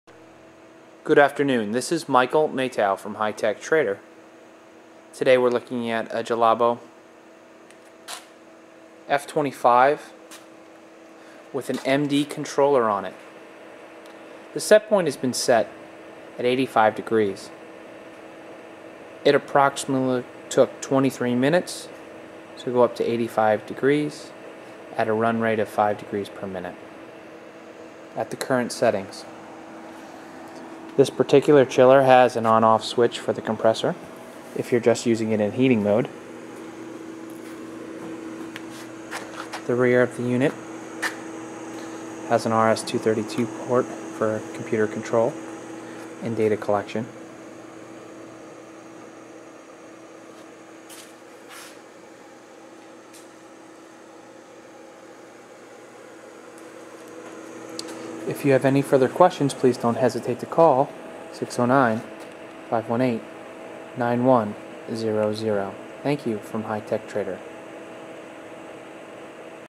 Julabo Chiller
Used lab equipment from HiTechTrader.com Julabo F25-MD Refrigerated Circulator Water Bath, Working temperature range -32 to 200 C; Temperature stability +/-0.01 C;